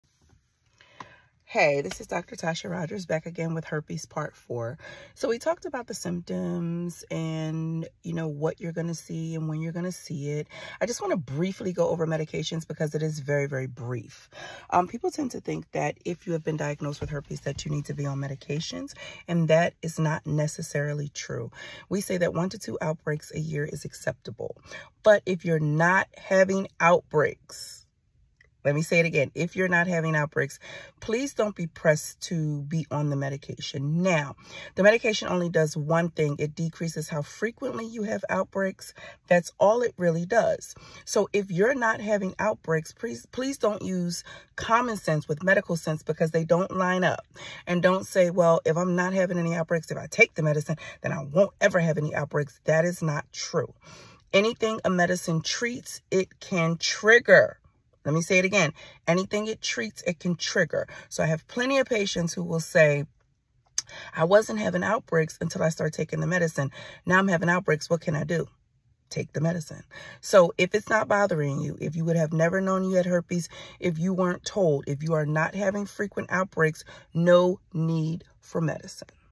All about herpes... Part 4
A series dedicated to dispelling the rumors, myths and lies about herpes. Real facts to educate folks, kill the stigma and stop the lies and hurt associated with having herpes.